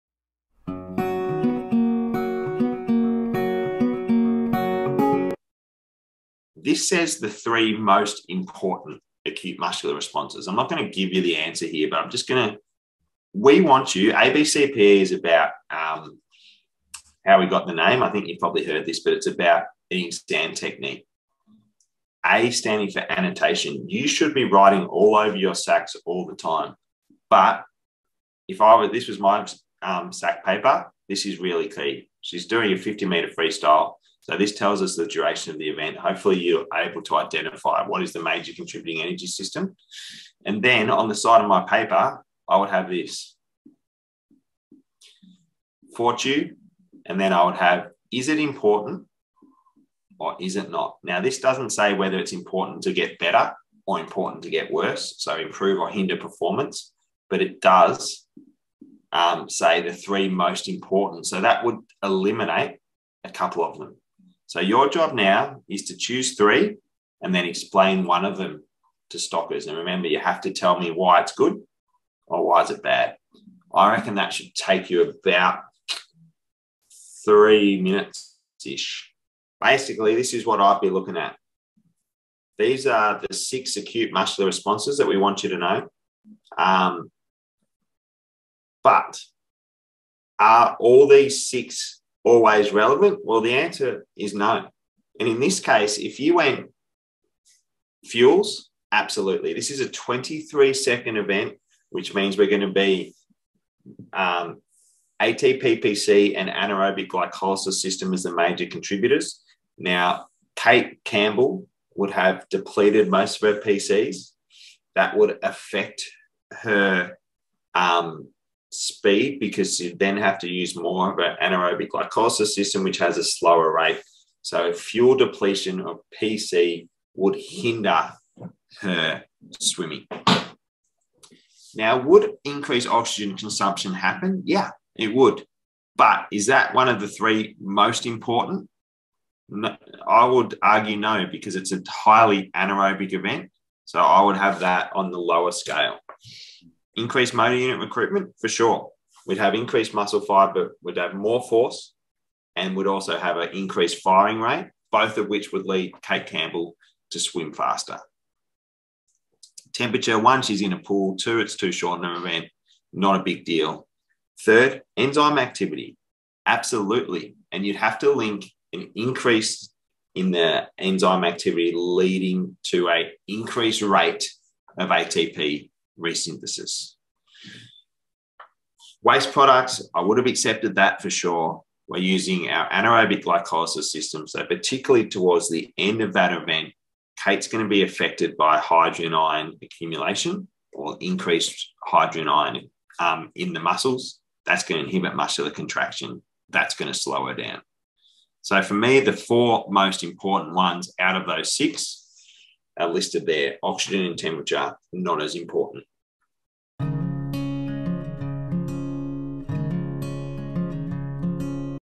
abcPE mini FORTEW (muscular acute responses!)
Hi guys- In this one we talk about how we think the best way to answer acute muscular response questions is! We hope it makes things as easy as!